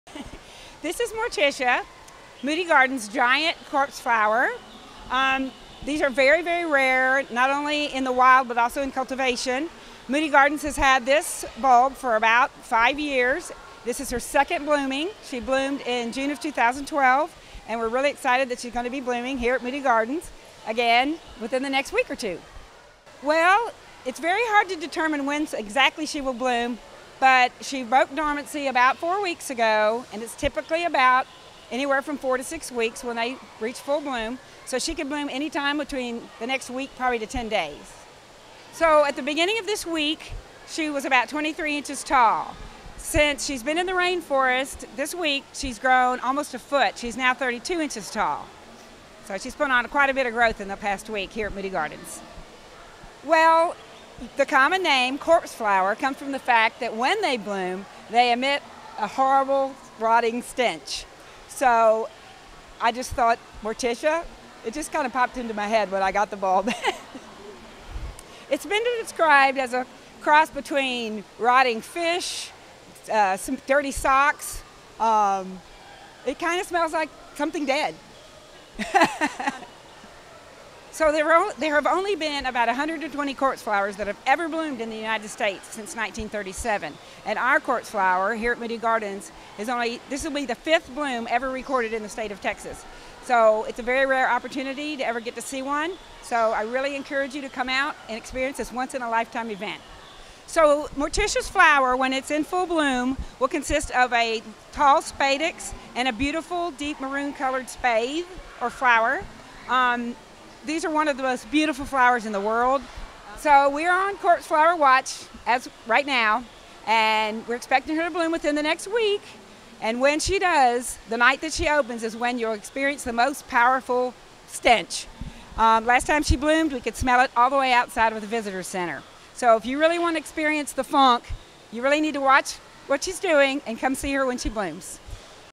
Morticia the Corpse Flower at Moody Gardens - Donita Brannon Interview
Moody Gardens is excited to announce Morticia, the Corpse Flower, who is expected to bloom within the next 10 days. Giant Corpse Flowers are aptly named for the pungent odor emitted during their rare blooms.